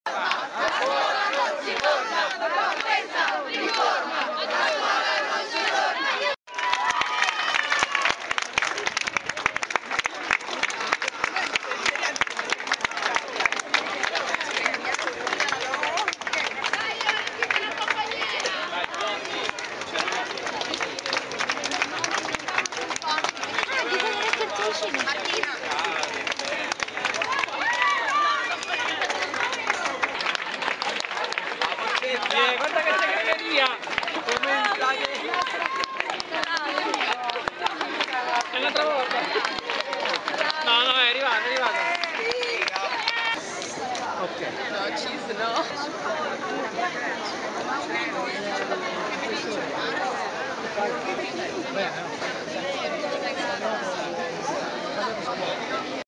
Studenti in protesta sfilano a Trinità dei Monti
Sulla scalinata di Trinità dei Monti sfila la protesta di studenti ed insegnanti. Sono i ragazzi e i docenti del Liceo Artistico Enzo Rossi del Tiburtino che hanno organizzato una manifestazione lampo in pieno centro. Un cosidetto "flash mob", una risposta "artistica" alle politiche dei tagli del governo Monti. I motivi della protesta sono condivisi sia da studenti che da docenti.